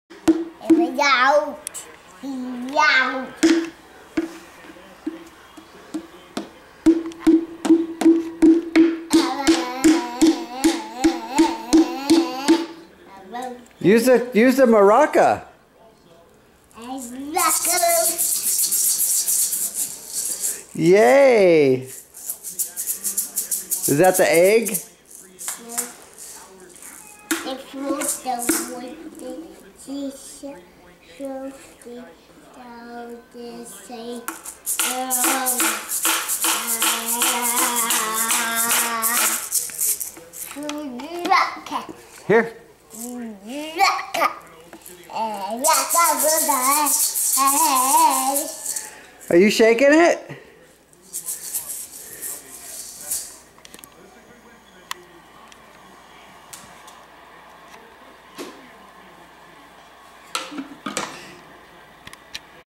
Finley Shakin Maraccas
Finley Having fun with her shakers and wood drum